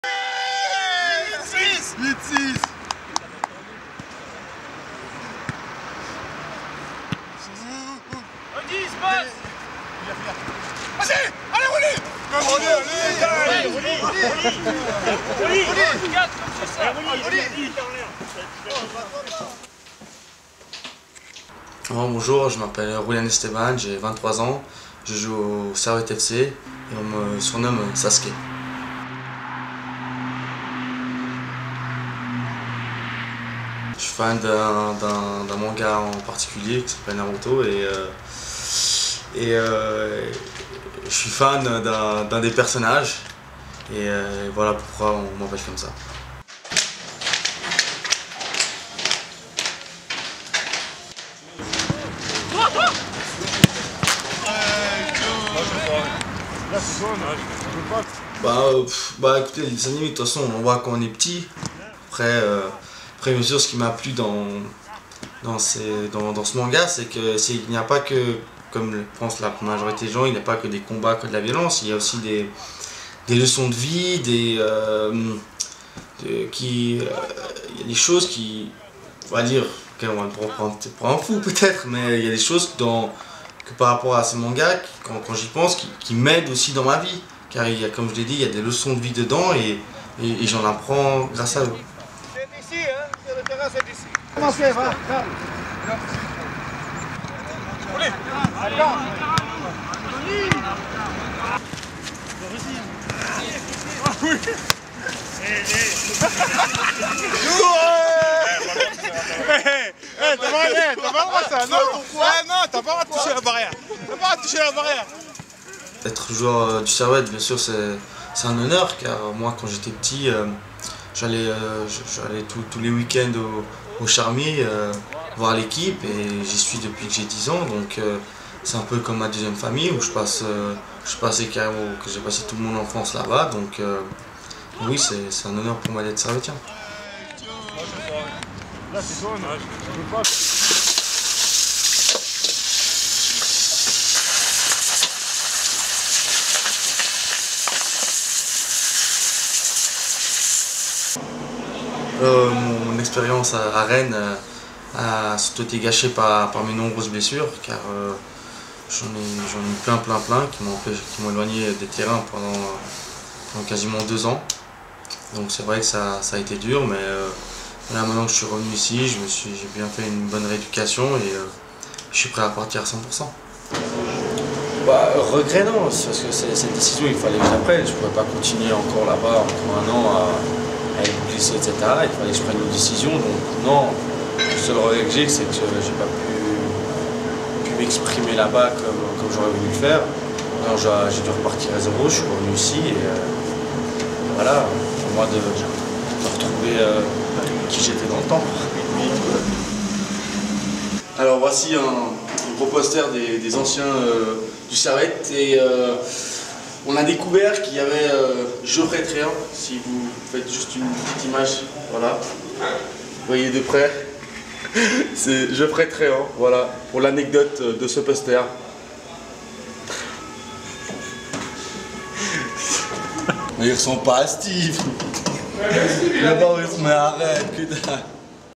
Julian Esteban
Buteur du Servette FC en 2006, le jeune homme de 23 ans, fan de mangas, est de retour dans son club formateur après une expérience mitigée en Ligue 1 française avec le Stade Rennais.